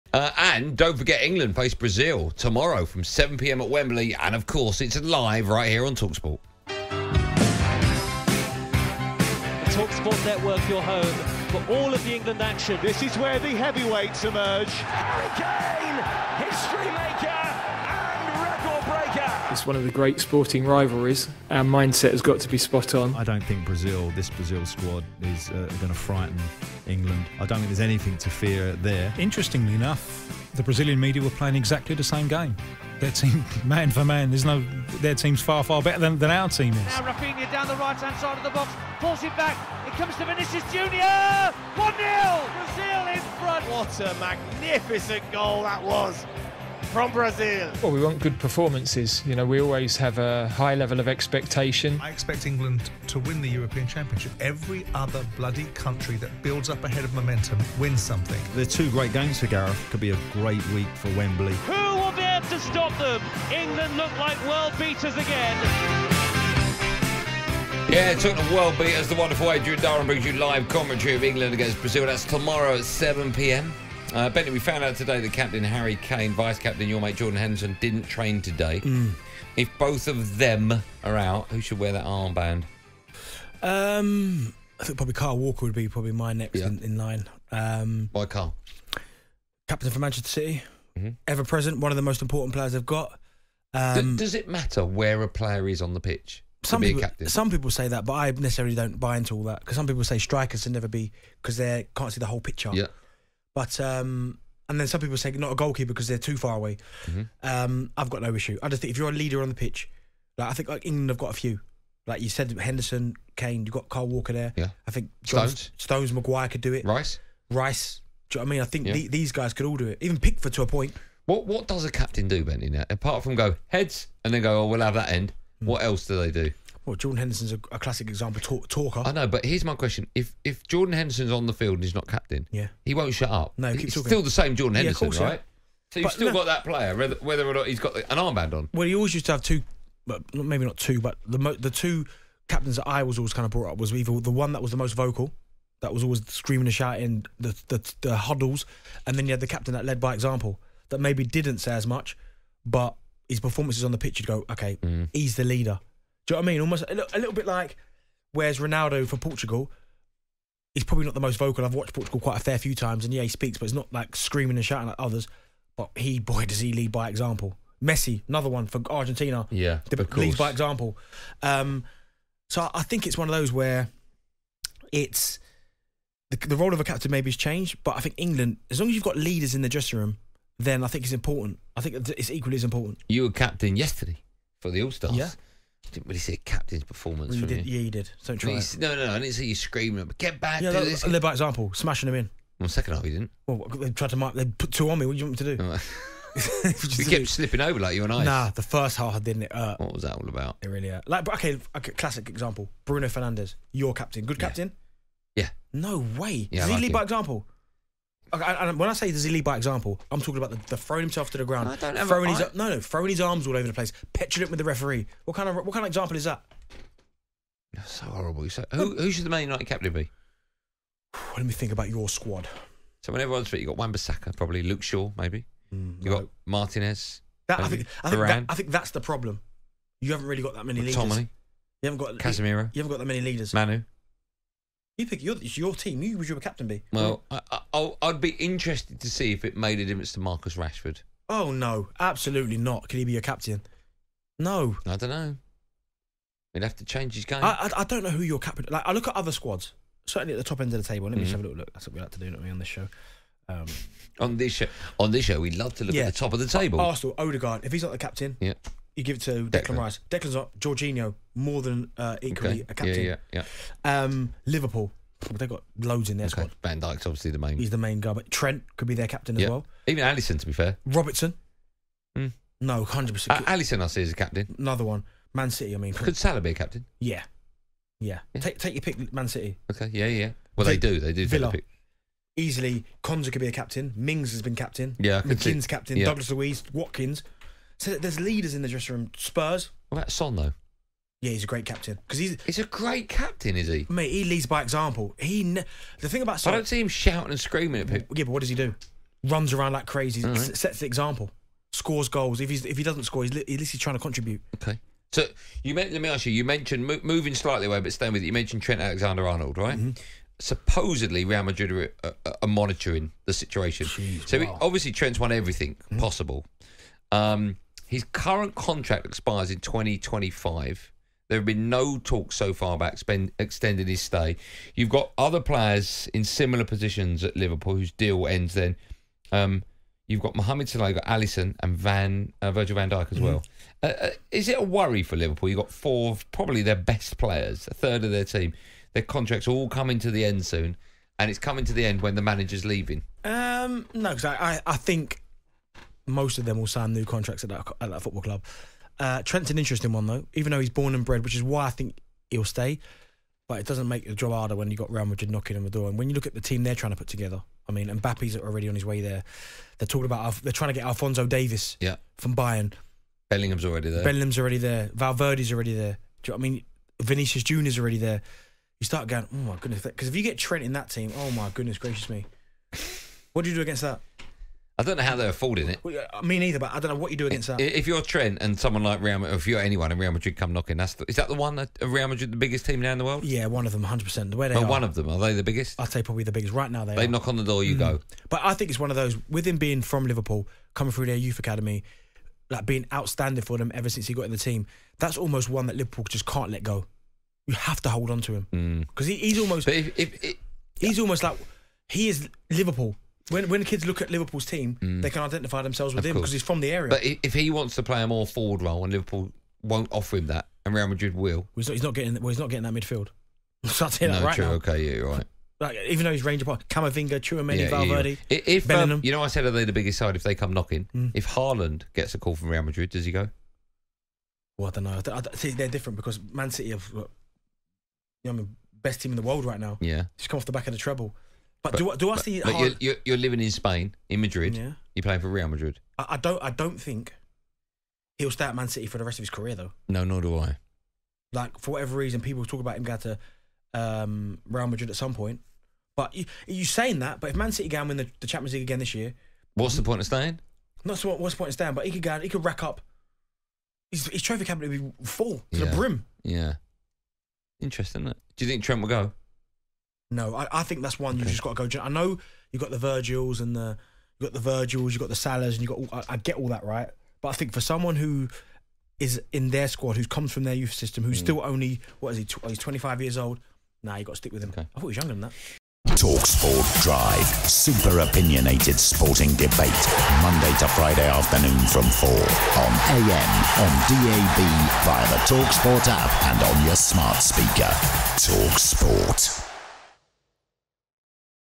Darren Bent BELIEVES Kyle Walker Is The Right Man To CAPTAIN England If Harry Kane Was Ever Out! 👀🤔
Darren Bent & Andy Goldstein discuss England and who should be their captain when going into the game.